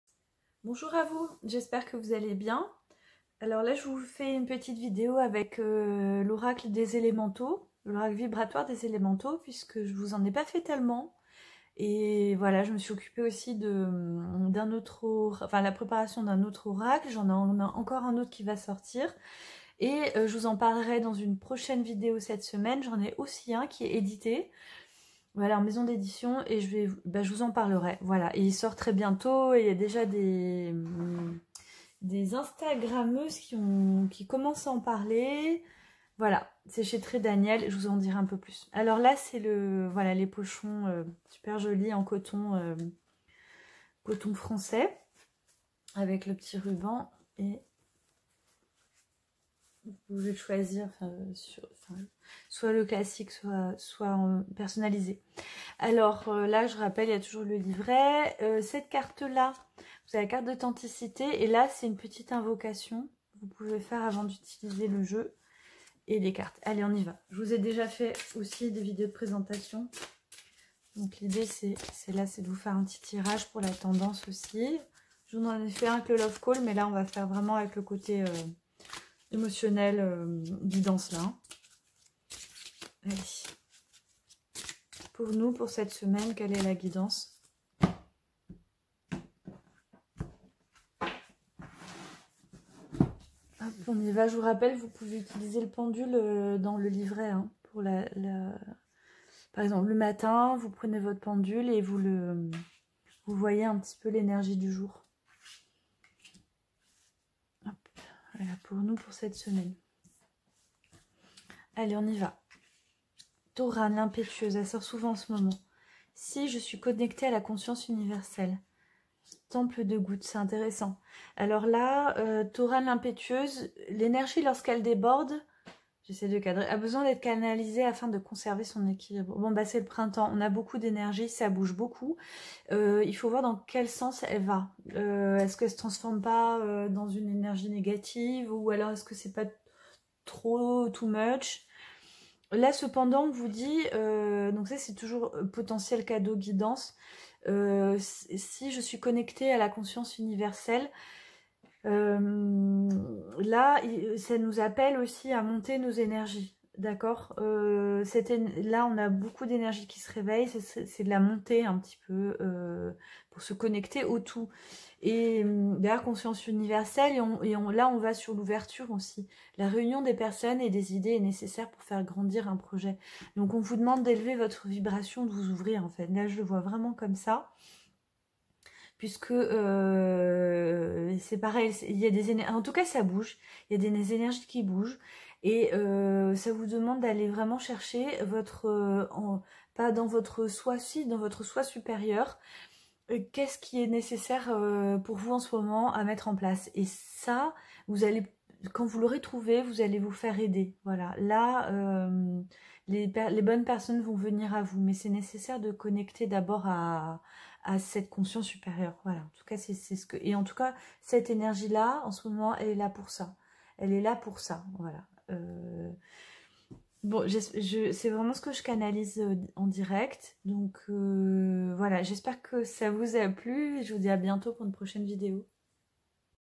Tirage de la semaine avec les Élément'eaux 💎💙🦋
💙 L'oracle vibratoire des Élément'Eaux 💙
L'oracle contient un livret et 40 cartes :
3 familles d'élément'eaux :
- les élément'eaux de l'Air
- les élément'eaux de la Mer
- les élément'eaux de la Terre
et les 7 notes de musique.
Le livret contient 32 pages avec l'explication de chaque carte, des tableaux, des conseils et une planche de radiesthésie au milieu.